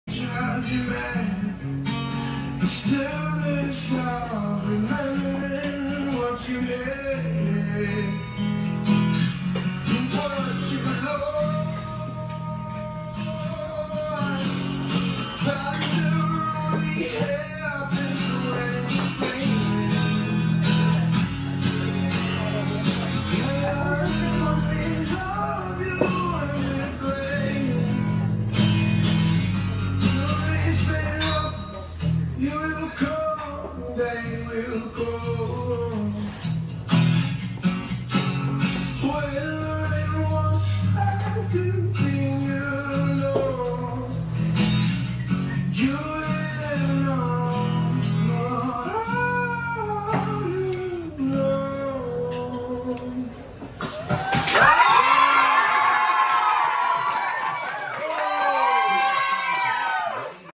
Frank Boone American Idol Watch Party Recap
The School of Rock Wake Forest Community got together to cheer on Frank Boone who teaches with us!